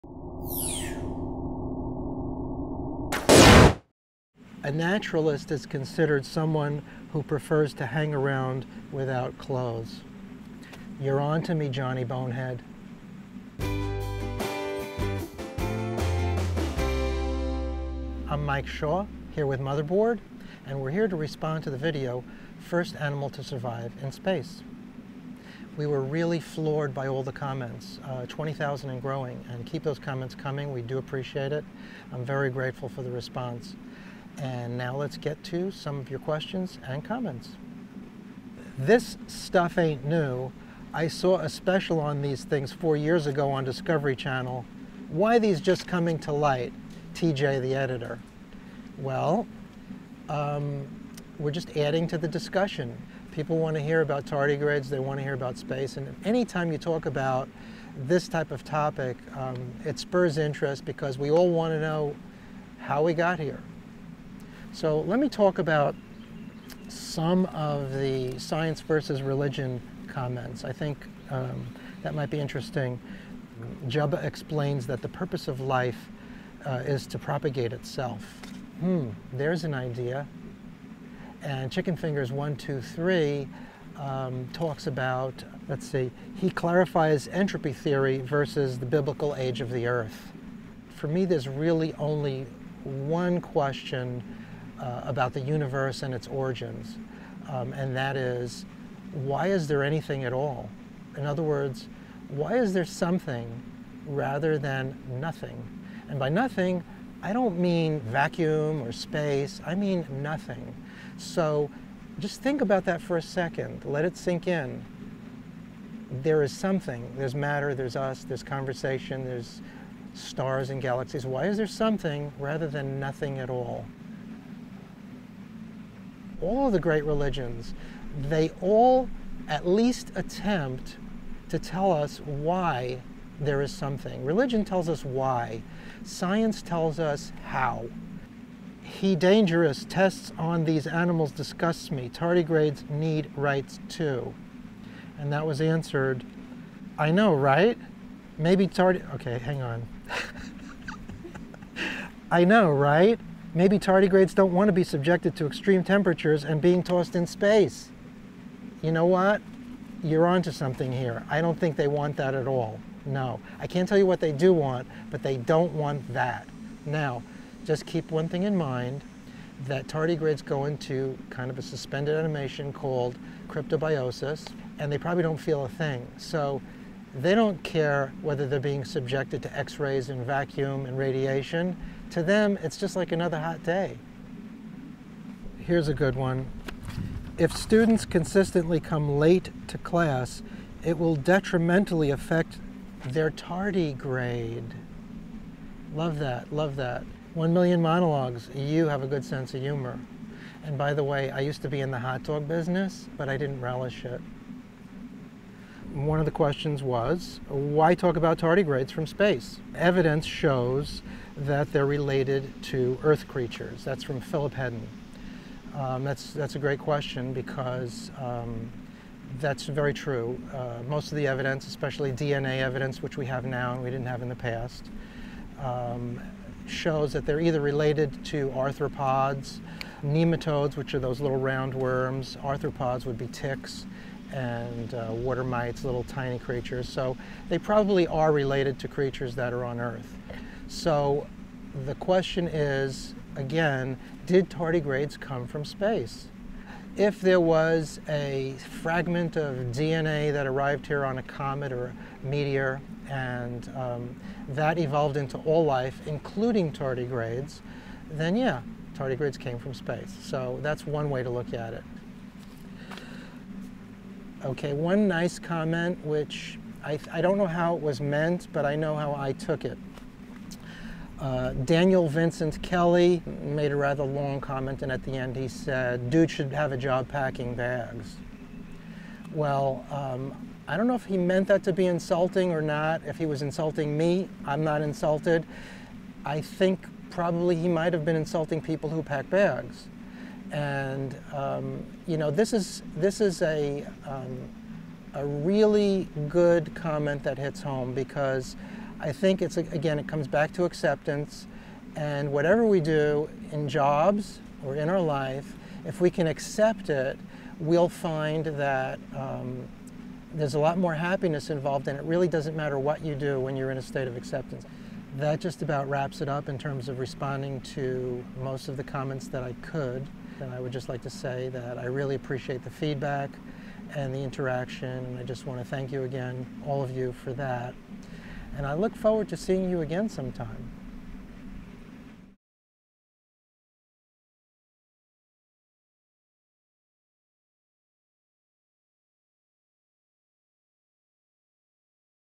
Comment Response: First Animal to Survive in Space
Science? Religion? Tardigrades? 42? Our piece received so much feedback that we had to ask Mike to respond to some of the most interesting, bizarre, and controversial comments.

"The First Animal to Survive in Space" struck a chord with the internet. Often, we here at Motherboard are not ever sure of why something pieces sail into the stratosphere while others remain cast away like an old teddy bear prize at Coney Island. With 9 million views and counting and over 20,000 comments Motherboard thought it behoved us to go down to Virginia once more and talk to the now internet-famous Mike Shaw. Out of the 20,000 comments there was a prevalent theme, the clash between the religious and the agonistic. Science vs. Faith. Mike Shaw contemplated this at national park in Virginia. He mused that science and religion both of a place in this video, that "religion tells us the why, science tells us the how." Mike spoke to these deep philosophical issues while still maintaining his wry sense of humor when it comes to often audacious YouTube commenters. Check out Motherboard's respond video here.